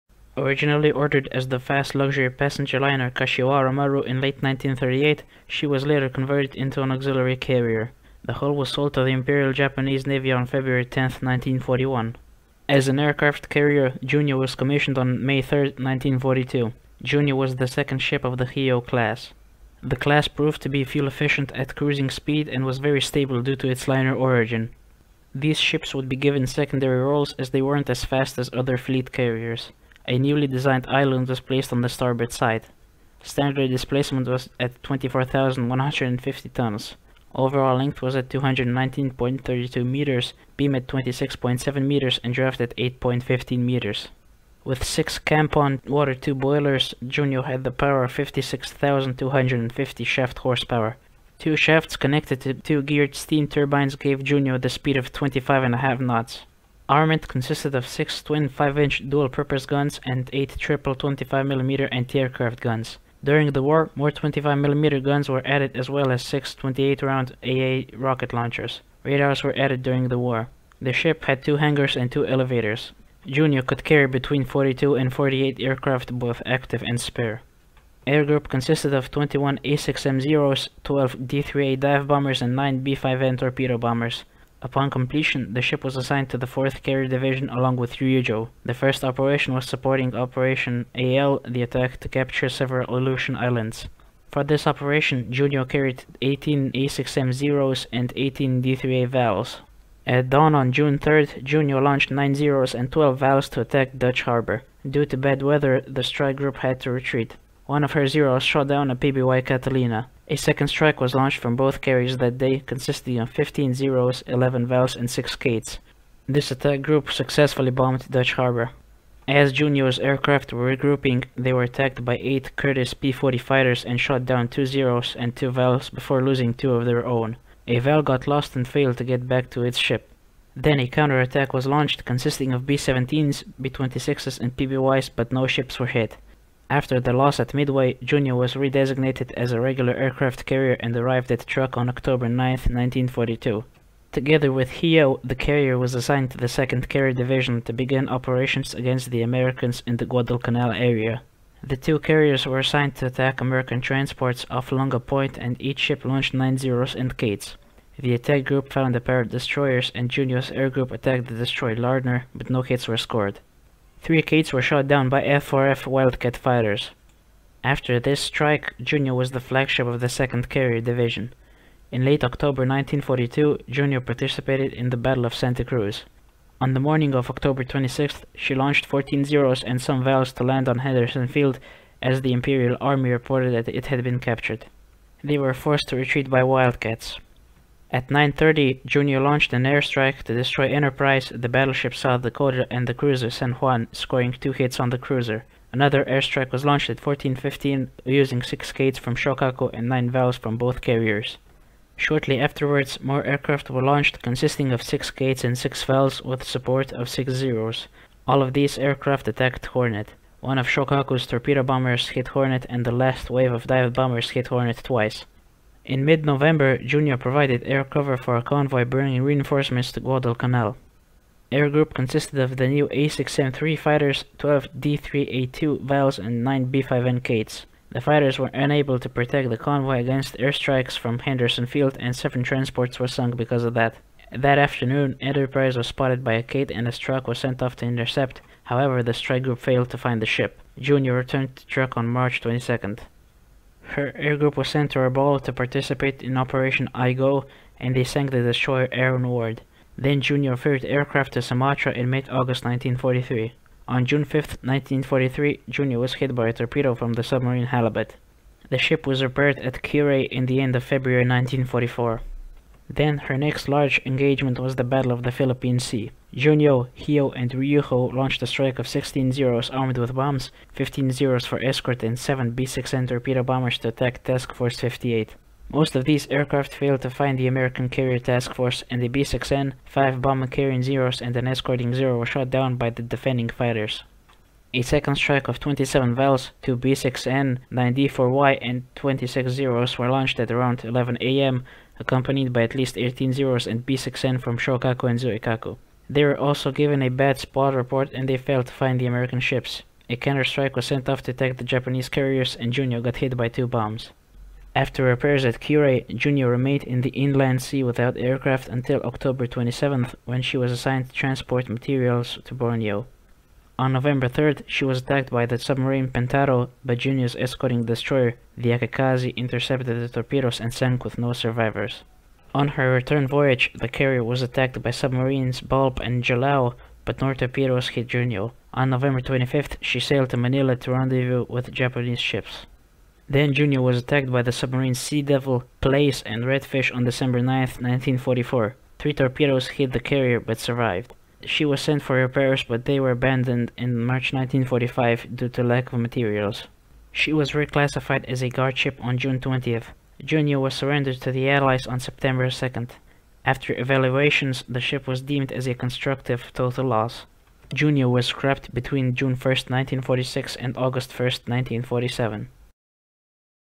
Junyo
This is a short video about the aircraft carrier Junyo, its design, armament and operational history.